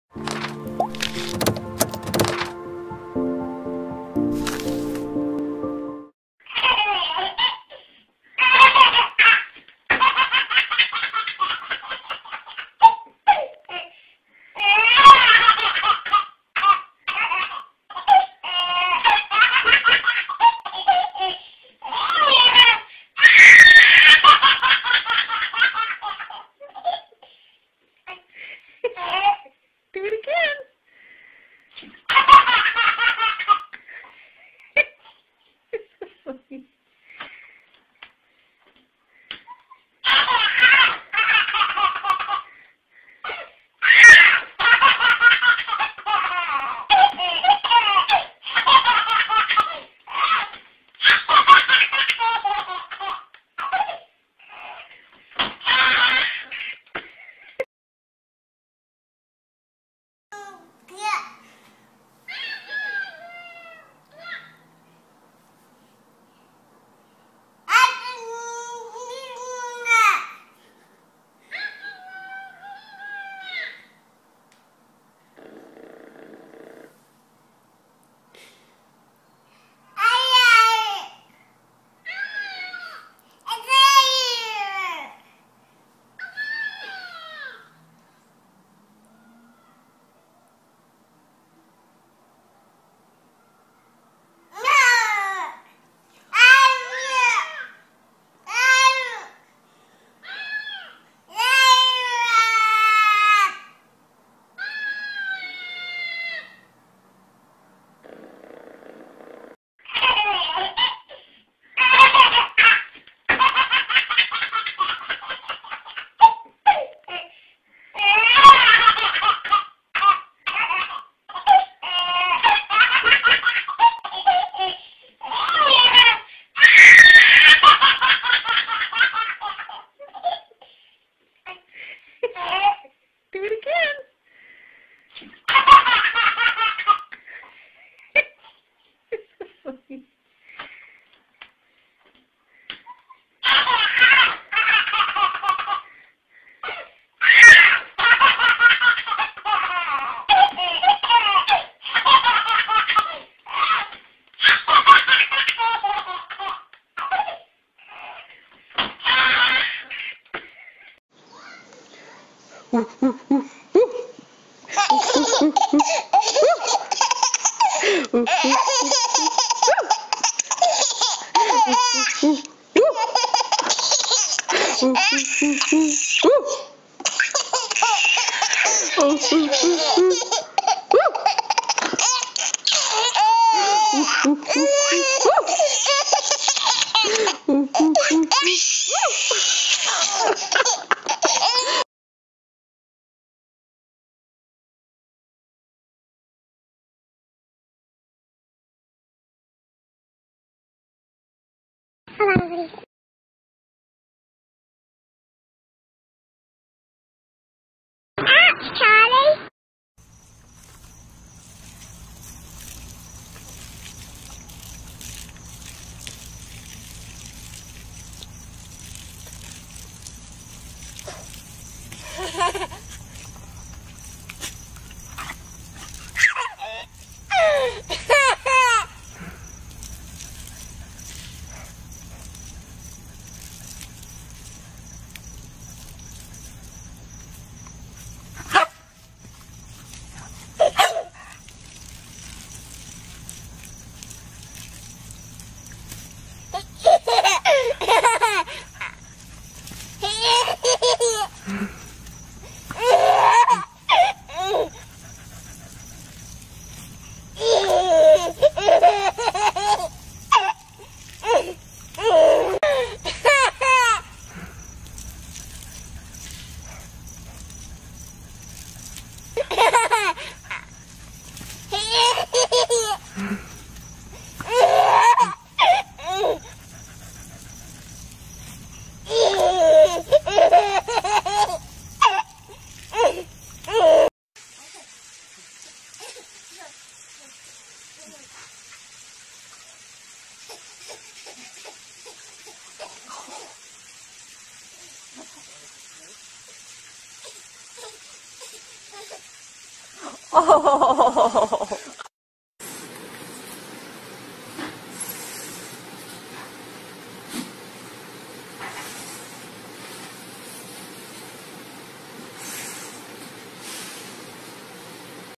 Babe playing and laughing with dogs
The babe having a good time with their dogs. Playing, Laughing